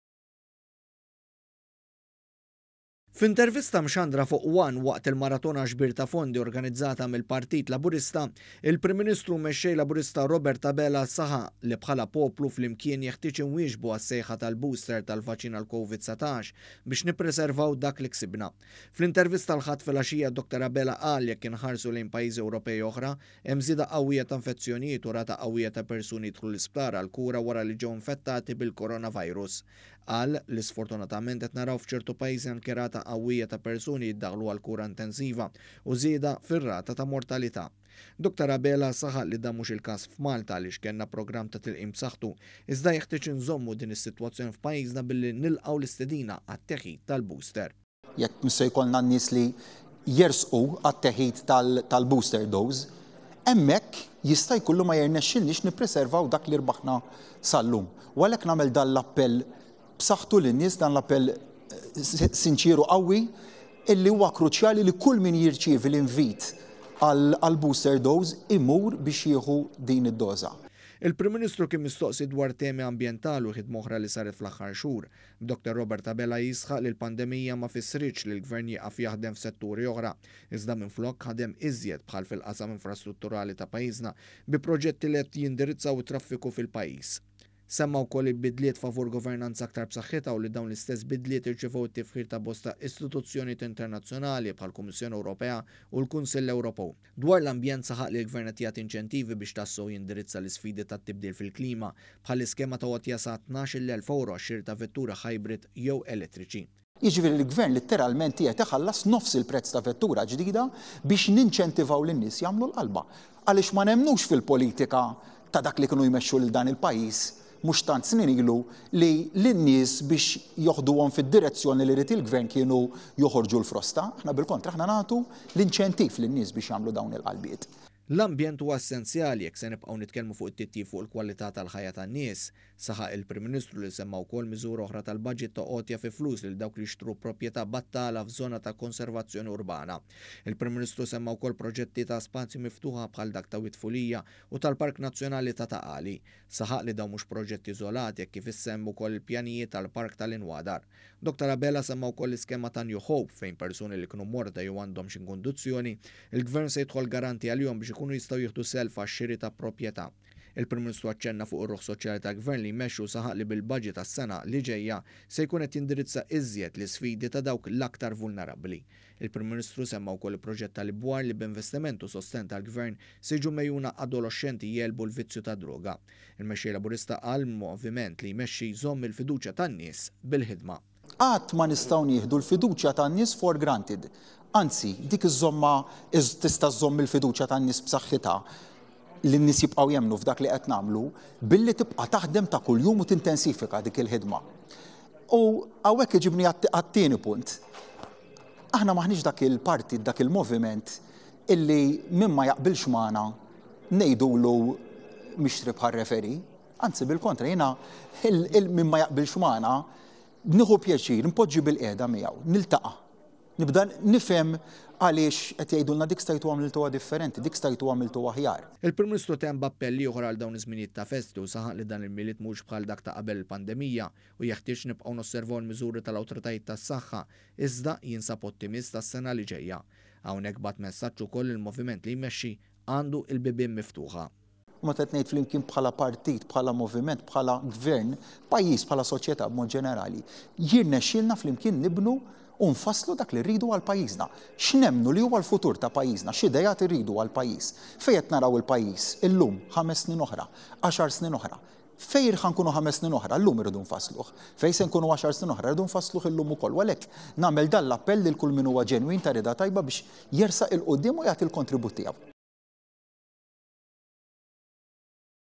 IL-PM JISĦAQ FUQ IT-TEĦID TAL-BOOSTER
ONE.COM.MT •

FILMAT            ONE / ARKIVJI

F’intervista mxandra fuq ONE waqt il-maratona ġbir ta’ fondi organizzata mill-Partit Laburista, il-Prim Ministru u Mexxej Laburista Robert Abela saħaq li bħala poplu, flimkien jeħtieġ inwieġbu għas-sejħa tal-booster tal-vaċċin għall-Covid-19, biex nippriservaw dak li ksibna.

Fl-intervista l-Ħadd filgħaxija, Dr Abela qal li jekk inħarsu lejn pajjiżi Ewropej oħra hemm żieda qawwija ta’ infezzjonijiet u rata qawwija ta’ persuni jidħlu l-isptar għall-kura wara li ġew infettati bil-Coronavirus.

Qal li sfortunatament qed naraw f’ċertu pajjiżi anki rata qawwija ta’ persuni jiddaħħlu għall-kura intensiva u żieda fir-rata ta’ mortalita.

Dr Abela saħaq li dan mhux il-każ f’Malta għaliex kellna programm ta’ tilqim b’saħħtu iżda jeħtieġ inżommu din is-sitwazzjoni f’pajjiżna billi nilqgħu l-istedina għat-teħid tal-booster.

SOT 1 ROBERT ABELA
            PRIM MINISTRU
“Jekk mhux se jkollna n-nies jersqu għat-teħid tal-booster dose, hemmhekk jista’ jkun li ma jirnexxilniex nippriservaw dak li rbaħna sal-lum u għalhekk nagħmel dan l-appell b’saħħtu lin-nies, dan l-appell sinċier u qawwi illi huwa kruċjali li kull min jirċievi l-invit għall-booster dose, imur biex jieħu din id-doża.”

Il-Prim Ministru kien mistoqsi dwar temi ambjentali u ħidma oħra li saret fl-aħħar xhur, b’Dr Robert Abela jisħaq li l-pandemija ma fissritx li l-Gvern jieqaf jaħdem f’setturi oħra iżda minflok ħadem iżjed bħal fil-qasam infrastrutturali ta’ pajjiżna bi proġetti li qed jindirizzaw it-traffiku fil-pajjiż.

Semma wkoll il-bidliet favur governanza aktar b’saħħitha u li dawn l-istess bidliet irċievew it-tifħir ta’ bosta istituzzjonijiet internazzjonali bħall-Kummissjoni Ewropea u l-Kunsill Ewropew.

Dwar l-ambjent saħaq li l-Gvern qed jagħti inċentivi biex tassew jindirizza l-isfidi tat-tibdil fil-klima bħall-iskema ta’ għotja sa €12,000 għax-xiri ta’ vetturi hybrid jew elettriċi.

 ROBERT ABELA
            PRIM MINISTRU
“Jiġifieri l-Gvern litteralment qiegħed iħallas nofs il-prezz ta’ vettura ġdida biex jinċentivaw lin-nies jagħmlu l-qalba għaliex ma nemmnux fil-politika ta’ dak li kienu jmexxu lil dan il-pajjiż mhux tant snin ilu li lin-nies biex joħduhom fid-direzzjoni li jrid il-Gvern kienu joħorġu l-frosta. Aħna bil-kontra, aħna nagħtu l-inċentiv lin-nies biex jagħmlu dawn il-qalbiet.”

“L-ambjent huwa essenzjali jekk se nibqgħu nitkellmu fuq it-titjib fuq il-kwalità tal-ħajja tan-nies”, saħaq il-Prim Ministru li semma wkoll miżura oħra tal-Budget ta’ għotja fi flus lil dawk li jixtru proprjetà battala f’żona ta’ konservazzjoni urbana.

Il-Prim Ministru semma wkoll proġetti ta’ spazji miftuħa bħal dak ta’ Wied Fulija u tal-Park Nazzjonali ta’ Ta’ Qali.

Saħaq li dawn mhux proġetti iżolati hekk kif issa hemm ukoll il-pjanijiet għall-Park tal-Inwadar.

Dr Abela semma wkoll l-iskema ta’ New Hope fejn persuni li kienu morda jew għandhom xi kundizzjoni, il-Gvern se jidħol garanti għalihom biex ikunu jistgħu jieħdu self għax-xiri ta’ proprjetà.

Il-Prim Ministru aċċenna fuq ir-ruħ soċjali tal-Gvern li jmexxi u saħaq li bil-Budget għas-sena li ġejja se jkun qed jindirizza iżjed l-isfidi ta’ dawk l-aktar vulnerabbli.

Il-Prim Ministru semma wkoll il-proġett tal-Ibwar, li b’investiment u sostenn tal-Gvern, se jiġu mgħejjuna adoloxxenti jegħlbu l-vizzju tad-droga.

Il-Mexxej Laburista qal li l-moviment li jmexxi jżomm il-fiduċja tan-nies, bil-ħidma.

ROBERT ABELA
            PRIM MINISTRU
“Qatt ma nistgħu nieħdu l-fiduċja tan-nies for granted anzi dik tista’ iżżomm il-fiduċja tan-nies b’saħħitha, lin-nies jibqgħu jemmnu f’dak li qed nagħmlu billi tibqa’ taħdem ta’ kuljum u tintensifika dik il-ħidma u hawnhekk niġi għat-tieni punt. Aħna m’aħniex dak il-partit, dak il-moviment illi min ma jaqbilx magħna ngħidulu mixtri bħar-referee. anzi bil-kontra. Jiena min ma jaqbilx magħna nieħu pjaċir inpoġġu bilqiegħda niltaqa’, nibda nifhem għaliex dik stajtu għamiltuha differenti, dik stajtu għamiltuha aħjar.”

Il-Prim Ministru temm b’appell ieħor għal dawn iż-żminijiet ta’ festi u saħaq li dan il-Milied m’huwiex bħal dak ta’ qabel il-pandemija u jeħtieġ nibqgħu nosservaw il-miżuri tal-Awtoritajiet tas-Saħħa iżda jinsab ottimist għas-sena li ġejja.

Hawnhekk bagħat messaġġ ukoll li l-moviment li jmexxi għandu l-bibien miftuħa.

 ROBERT ABELA
            PRIM MINISTRU
“U meta qed ngħid flimkien bħala partit, bħala moviment, bħala Gvern. Pajjiż bħala soċjetà ġenerali, b’mod ġenerali, jirnexxielna flimkien nibnu u nfasslu dak li rridu għal pajjiżna. X’nemmnu li huwa l-futur ta’ pajjiżna, x’ideat irridu għall-pajjiż? Fejn qed naraw il-pajjiż illum, ħames snin oħra, għaxar snin oħra? Fejn ħa nkunu ħames snin oħra, illum irridu nfasslu. Fejn se nkunu għaxar snin oħra, irridu nfassluh illum u għalhekk nagħmel dan l-appell lil kull min huwa ġenwin, ta’ rieda tajba, biex jersaq ’il quddiem u jagħti l-kontribut tiegħu.”